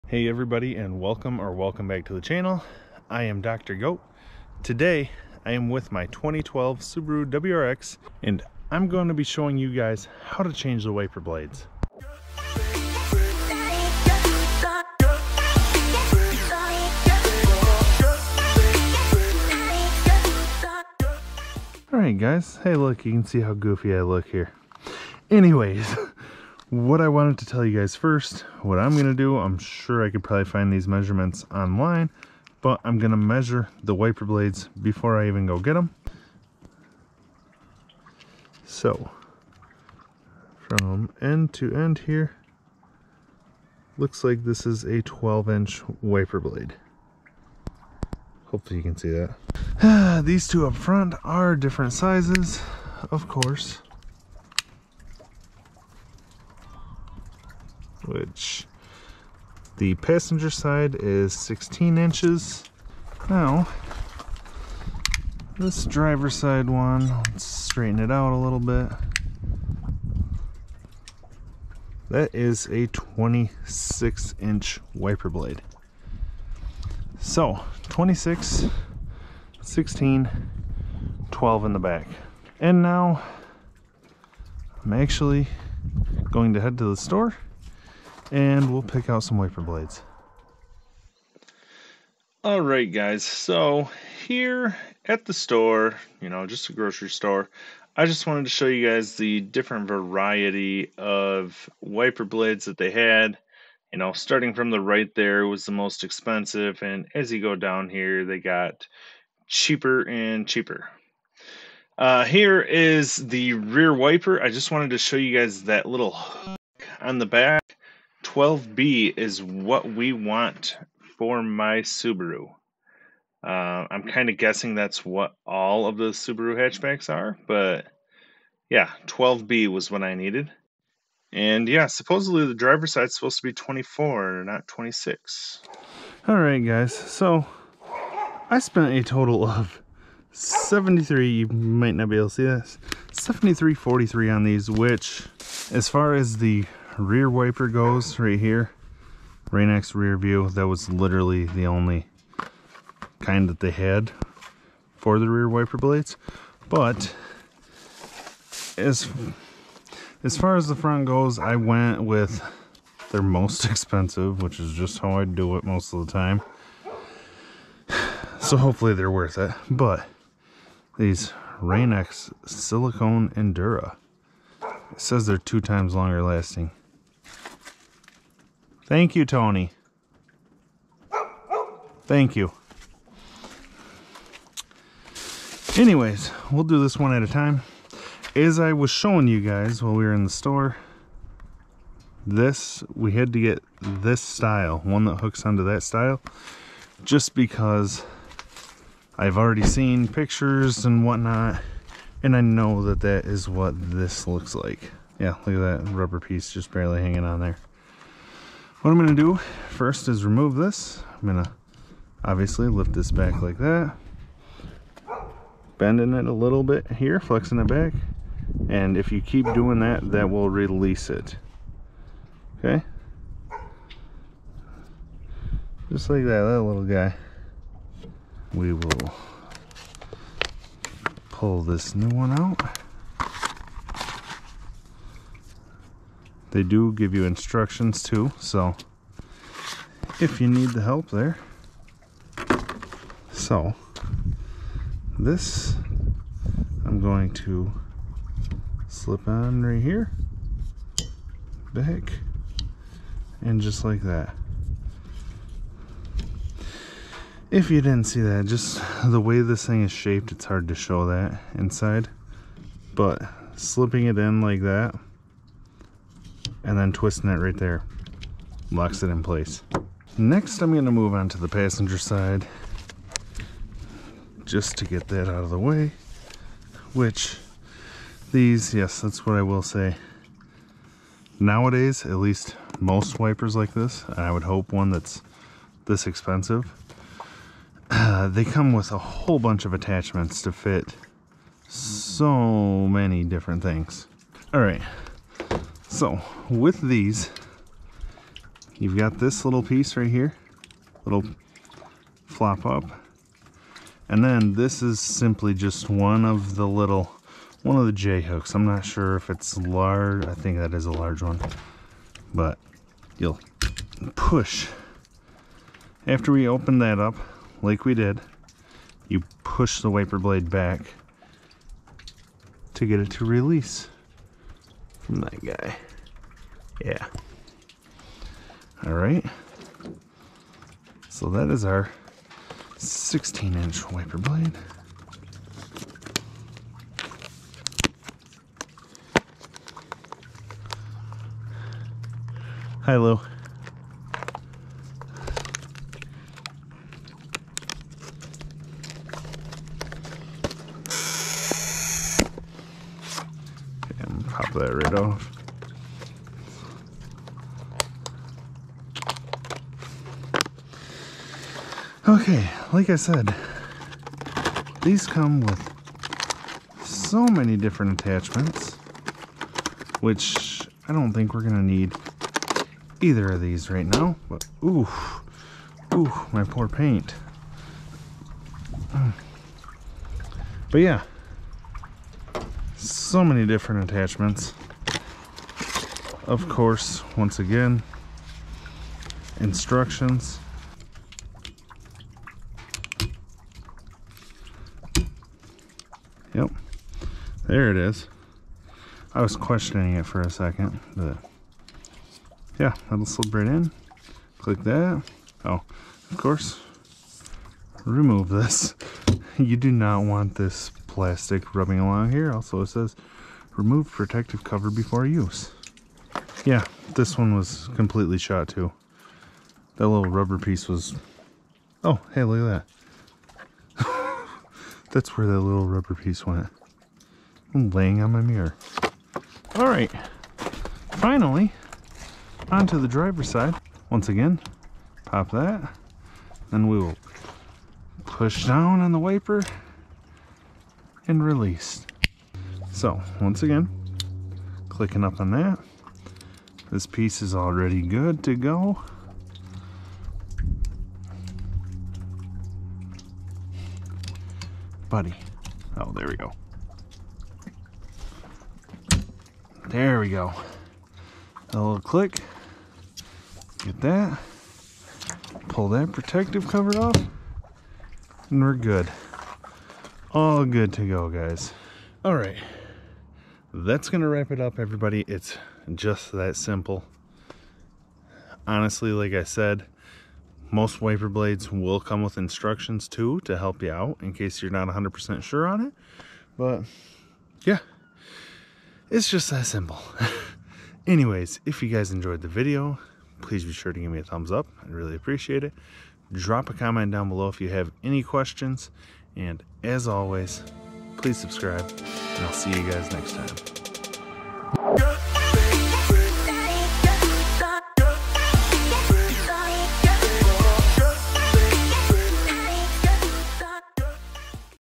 How To Replace Windshield Wiper Blades EASY! (Subaru WRX Hatchback)
How To Replace Windshield Wiper Blades EASY! (Subaru WRX Hatchback)//Today I'm finally replacing windshield wiper blades on my 2012 Subaru WRX Hatchback! Changing windshield wipers is honestly not that difficult to do and is a quick easy process! My Subaru WRX Hatchback has 3 different size wiper blades, including the back window, and I'm going to show you the sizes I got, and exactly how to replace windshield wiper blades in just a few minutes. Let me know what you guys want to see next!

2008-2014 Factory Hatchback Wiperblades (CHEAP!)